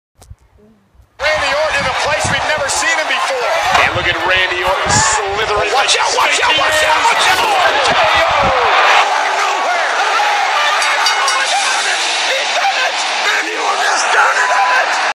RKO outta nowhere on my brother
Just a short video clip of me doing a RKO on my brother;)